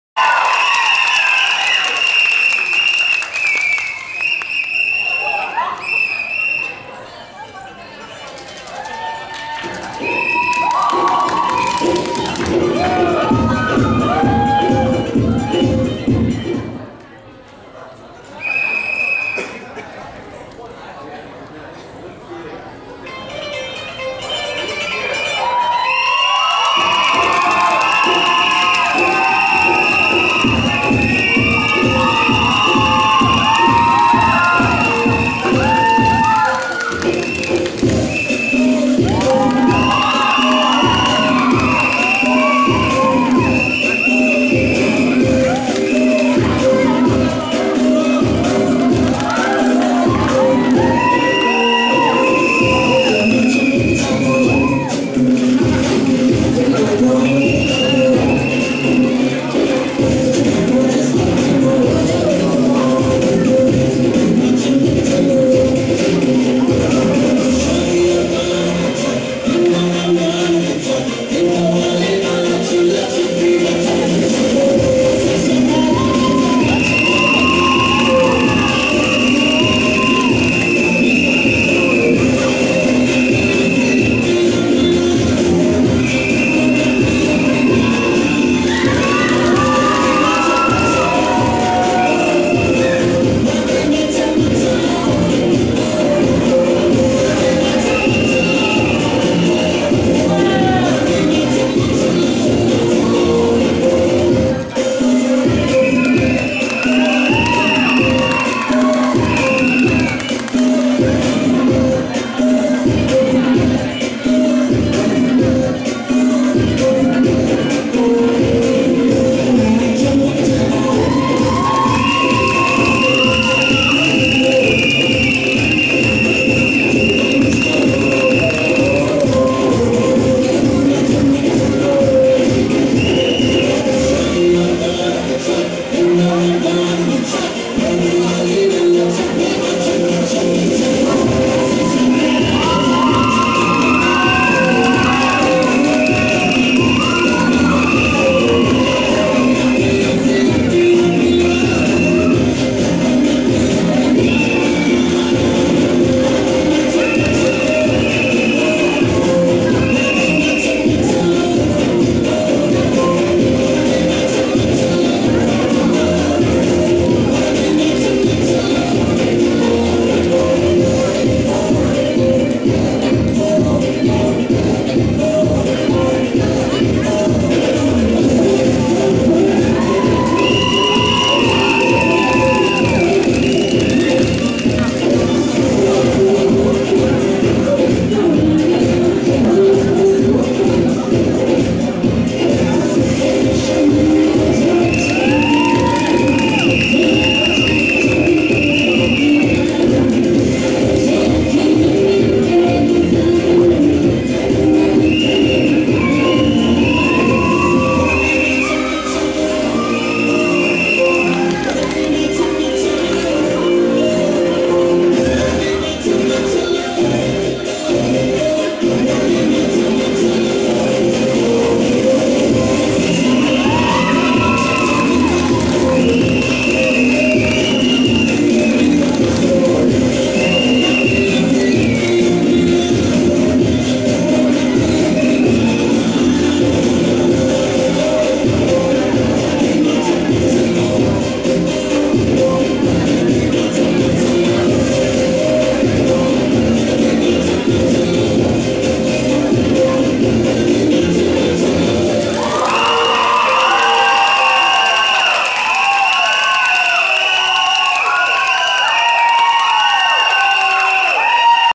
CRESCENDO INTERNATIONAL COLLEGE : INFINITY
CRESCENDO 10TH BIRTHDAY ANNIVERSARY CELEBRATION + COUNTDOWN
PERFORMANCE BY INFINITY